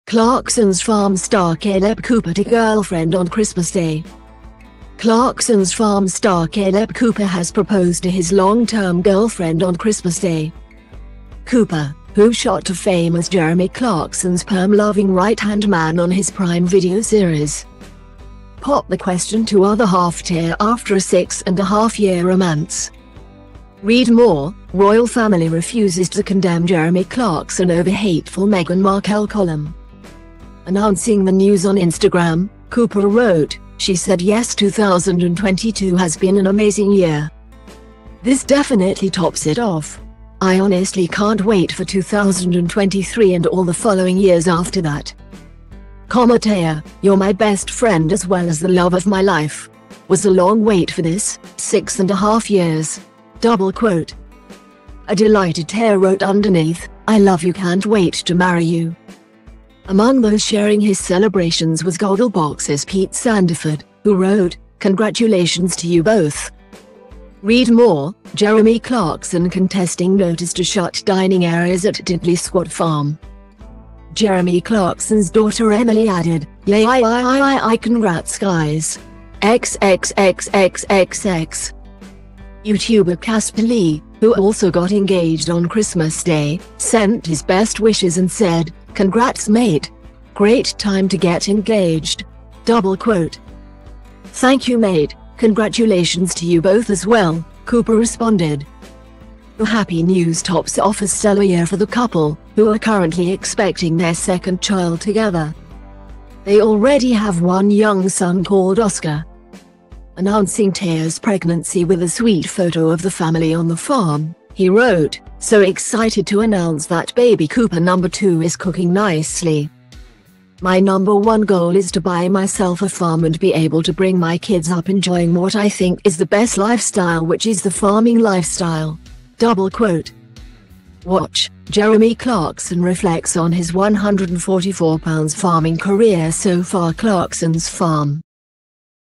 Clarkson's Farm star Kaleb Cooper to girlfriend on Christmas Day
Clarkson's Farm star Kaleb Cooper has proposed to his long-term girlfriend on Christmas Day. Cooper, who shot to fame as Jeremy Clarkson's perm-loving righthand man on his Prime Video series,popped the question to other half Taya after a six-and-a-half year romance.